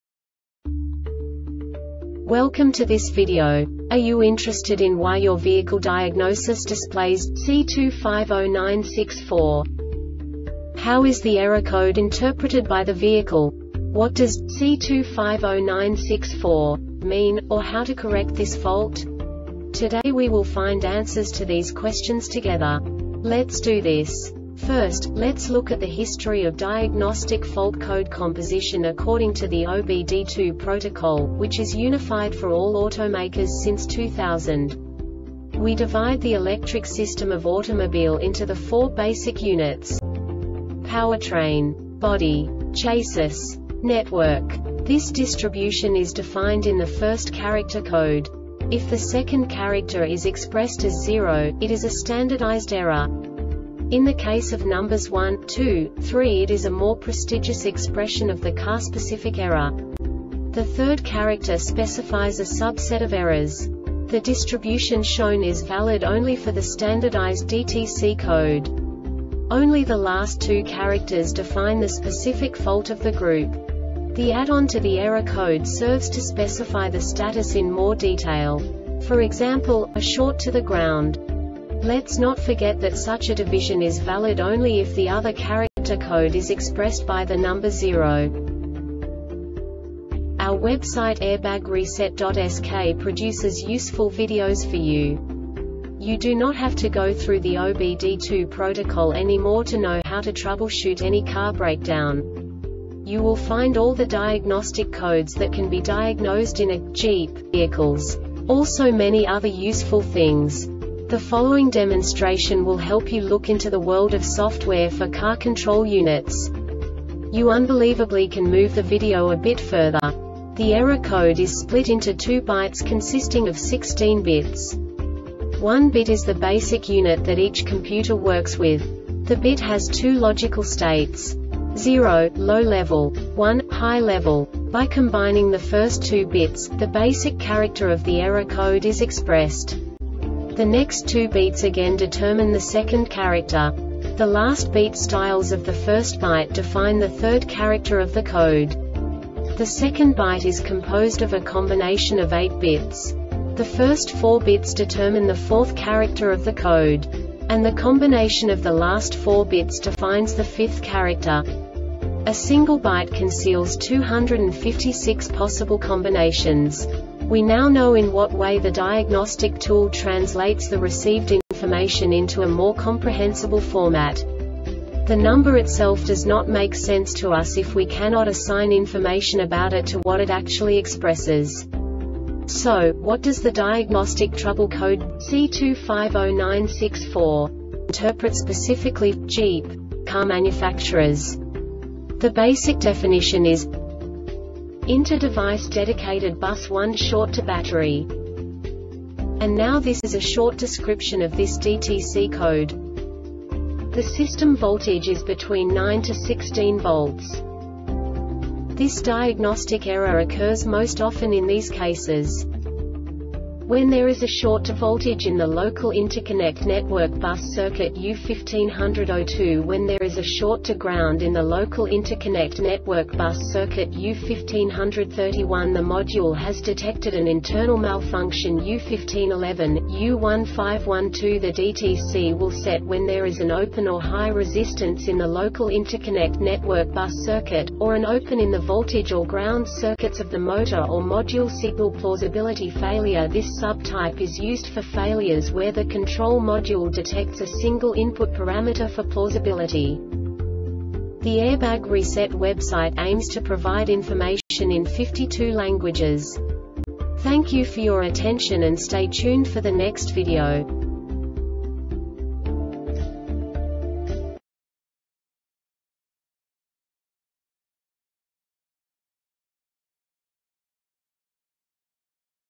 DTC Jeep C2509-64 Short Explanation
Contents:
0:21 Basic DTC analysis according to OBD2 protocol standard.
1:48 Insight into programming
2:58 A diagnostically understandable description of the Diagnostic Trouble Code
3:05 Basic error definition

Make
Jeep
Code
C2509 64
Definition
Inter-Device Dedicated Bus 1 Short to Battery
Description
The system voltage is between 9-16 V
Cause
When there is a short to voltage in the local interconnect network bus circuitU1500 02When there is a short to ground in the local interconnect network bus circuitU1500 31The module has detected an internal malfunctionU1511, U1512The DTC will set when there is an open or high resistance in the local interconnect network bus circuit,  or an open in the voltage or ground circuits of the motor or module
Failure Type